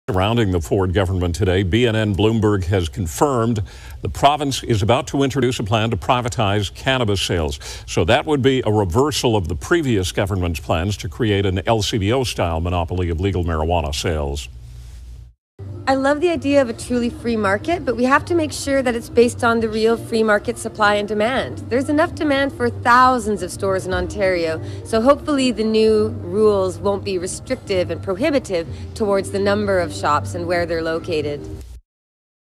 JODIE'S JOINT: "Jodie Emery reacts to reports of pot privatization"
CTV News Toronto interviewed Jodie Emery at "Jodie's Joint" hemp cafe about Ontario premier Doug Ford's announcement that private retail would take over cannabis sales.

Jodie Emery reacts to reports of pot privatization

The province is about to introduce a plan to privatize cannabis sales, according to reports, reversing the previous government's set up.